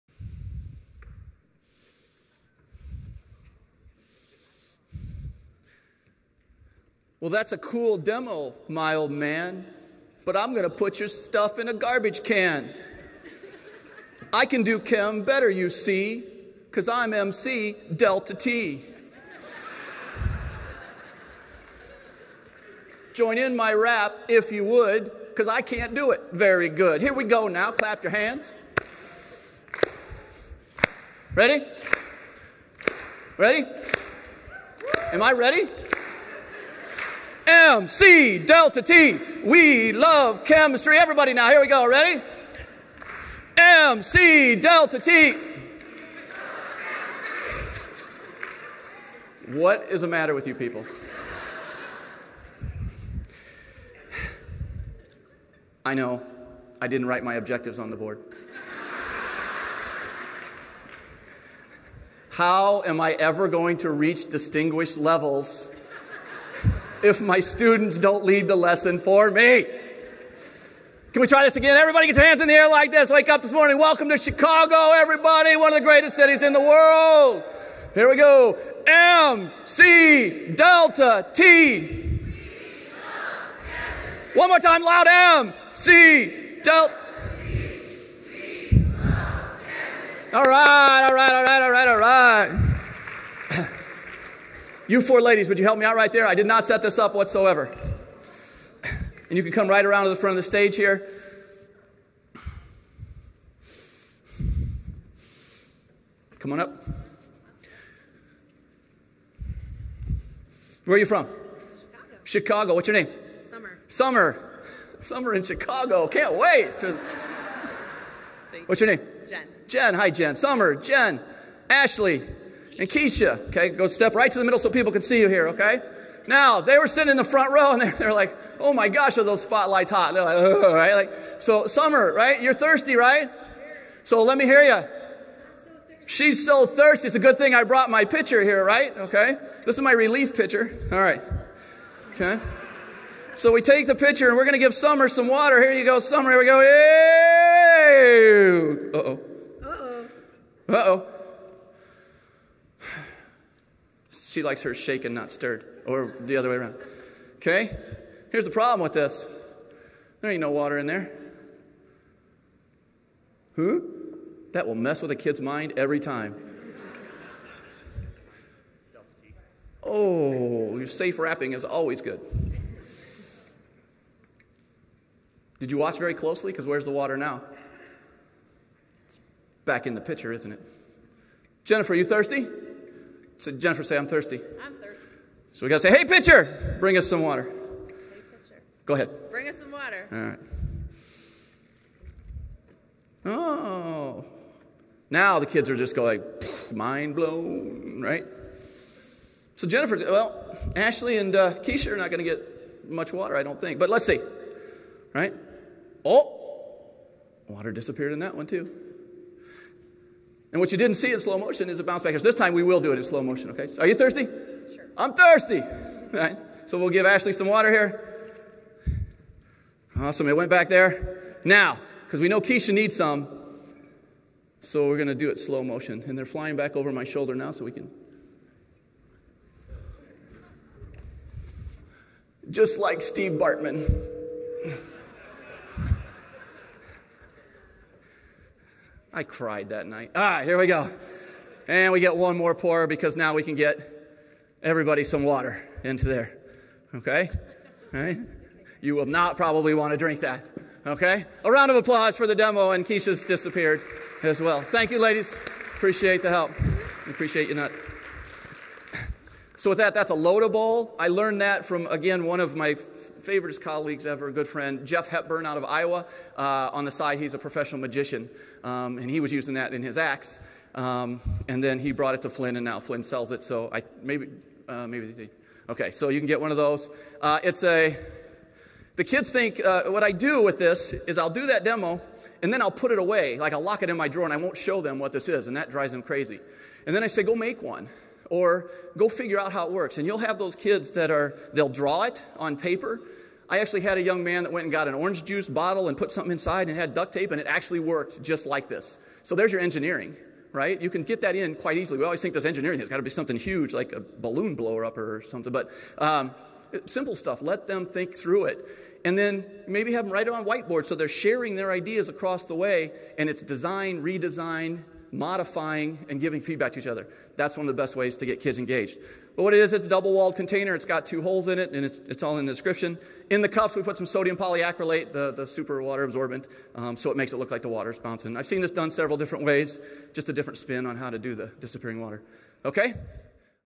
Lota Bowl with Sodium Polyacrylate Demonstration
Presented by: Mike Heinz – Downers Grove North High School

A container is filled with water. The water is poured out of the container until it appears empty. In a few minutes, more water naturally appears in the container and it is also poured out. The lota bowl is a classic part of many magic shows. A version of it has been around since the time of the ancient Greeks. The apparent “magical” properties of the container make it an excellent device to teach problem-solving in many science classes.